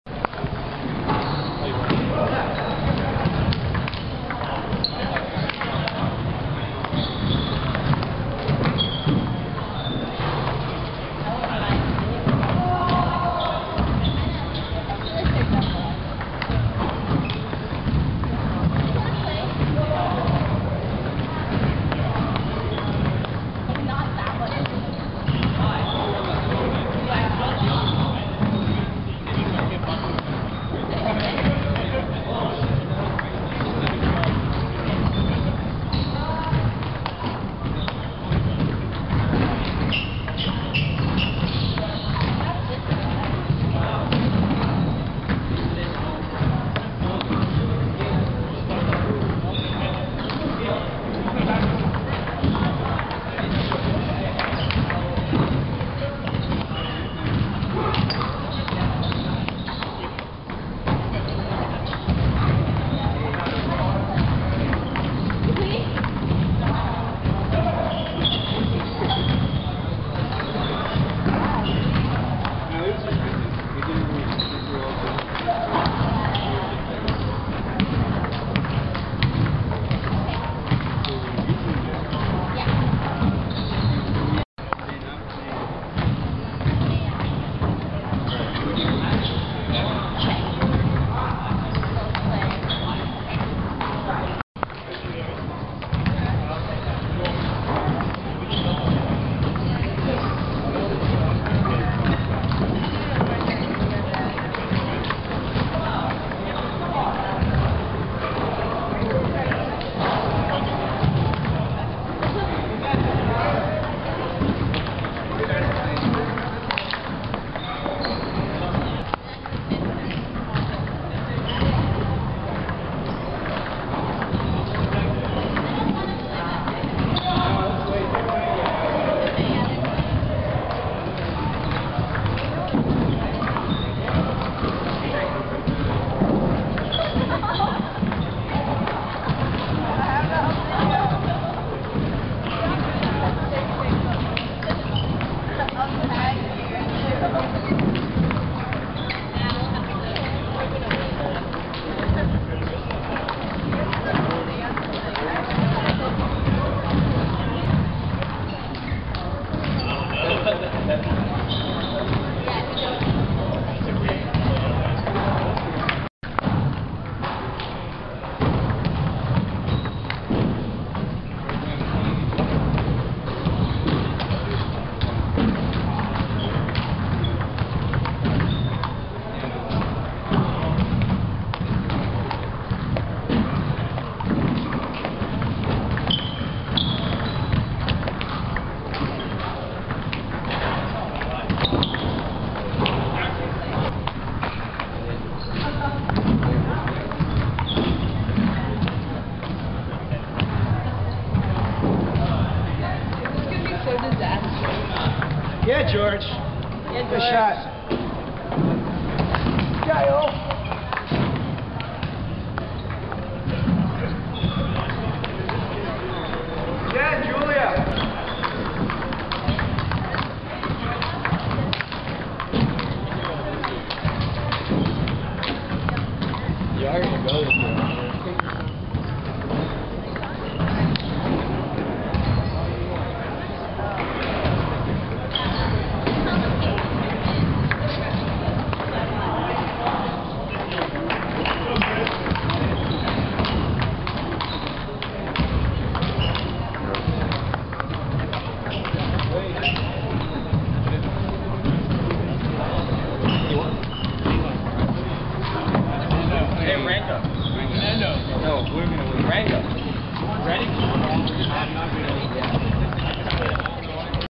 George Xing vs. Ranganathan Ramachandran
NCTTA mid-Atlantic division fall meet, Princeton University, Nov 8 2008
George Xing (Princeton) vs. Ranganathan Ramachandran (Penn)